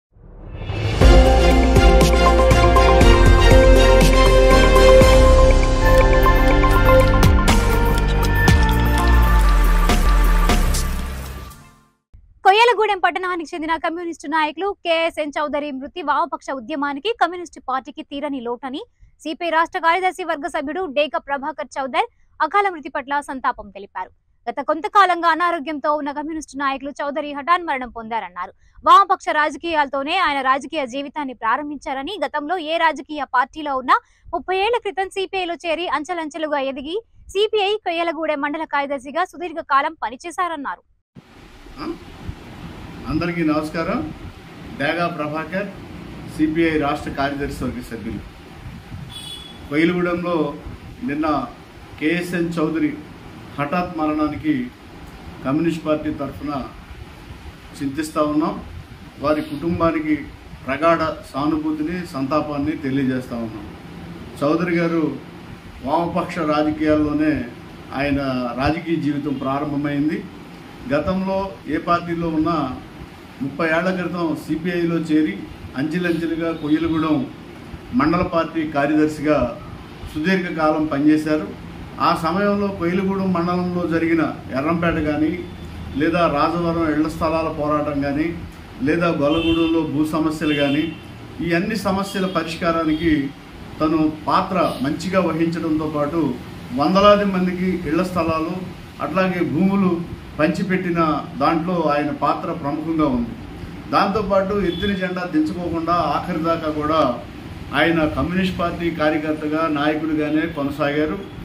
కమ్యూనిస్టు నాయకులు కె ఎస్ ఎన్ చౌదరి మృతి వామపక్ష ఉద్యమానికి ,కమ్యూనిస్టు పార్టీకి తీరని లోటనిmitv
కమ్యూనిస్టు నాయకులు కె ఎస్ ఎన్ చౌదరి మృతి వామపక్ష ఉద్యమానికి ,కమ్యూనిస్టు పార్టీకి తీరని లోటని